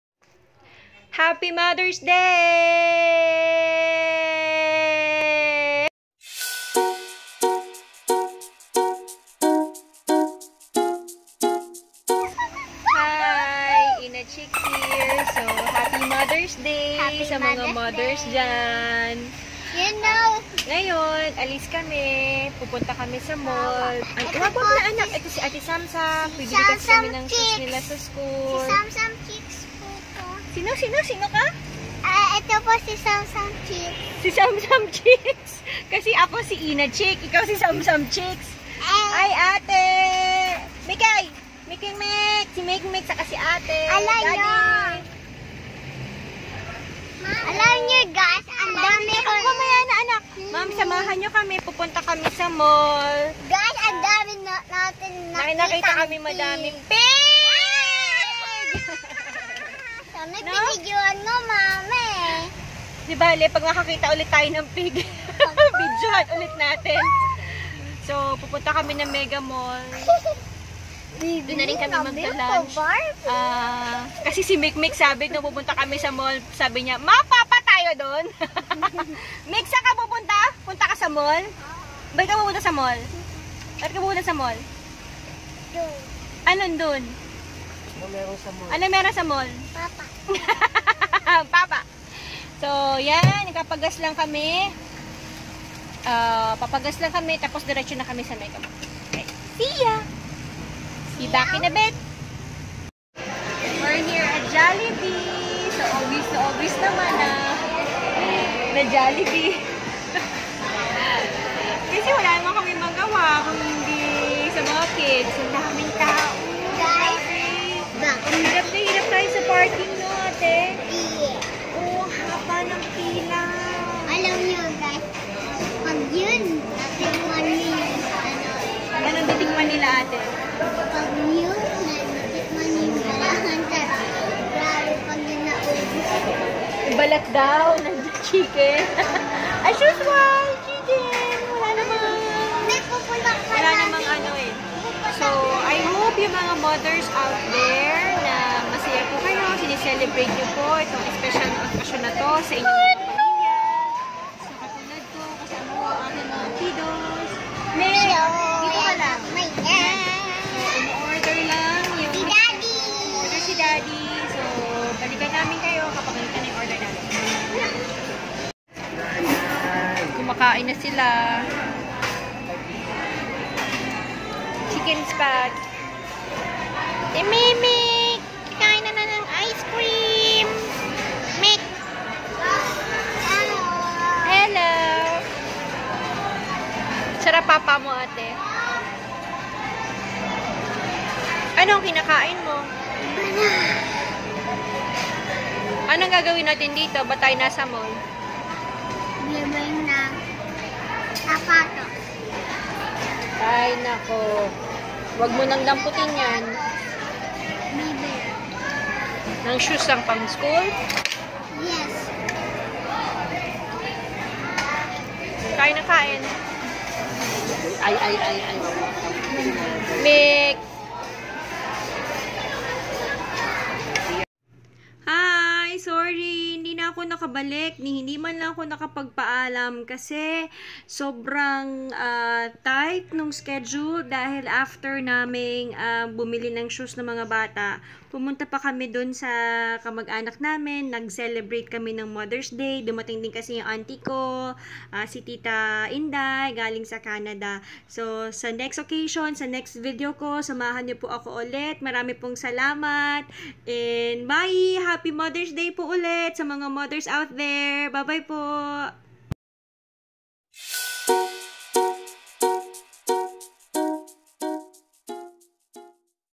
Mother's Day Celebration May 12, 2019
Hi guys, happy mother's day sa lahat po ng nanay sa buong mundo. Simple celebration with the family.